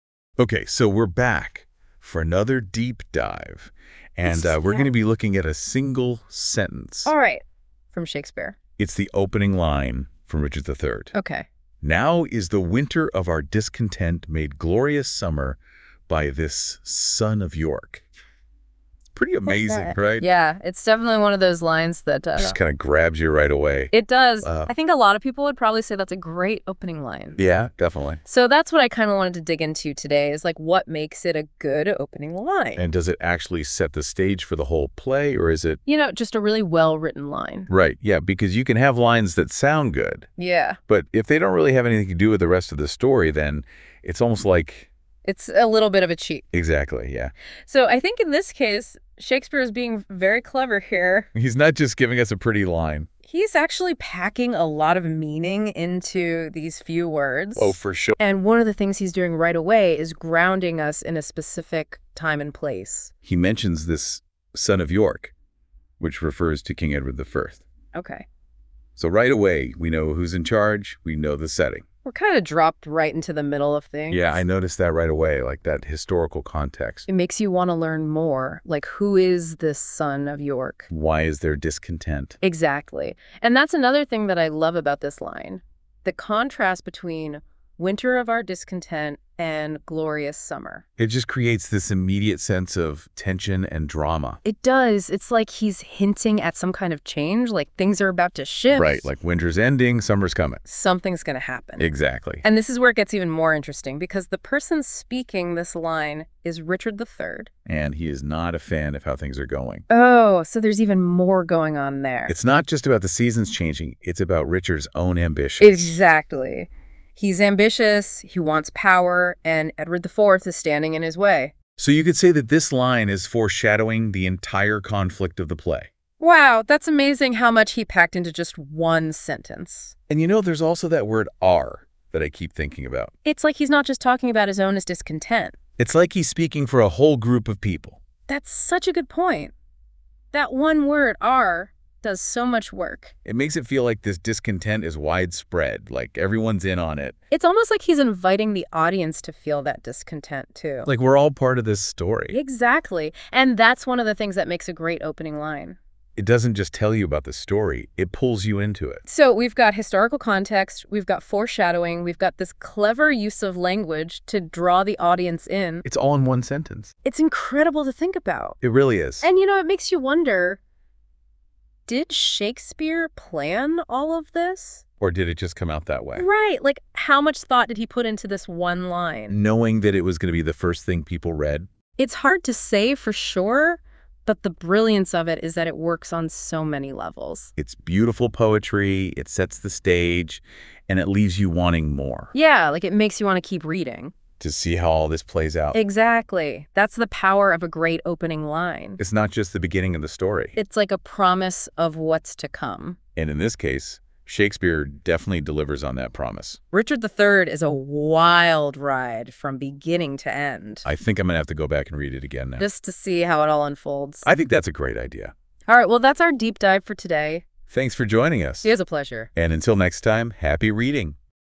Hooked from the First Line - Richard III
In this episode of Hooked from the First Line, we examine the iconic opening of Shakespeare’s Richard III: “Now is the winter of our discontent made glorious summer by this sun of York.” This line sets the tone for Richard’s ambition and dissatisfaction. Join us as we explore how Shakespeare uses metaphor, wordplay, and poetic style to introduce Richard’s complex character and foreshadow the play’s themes of power and ambition.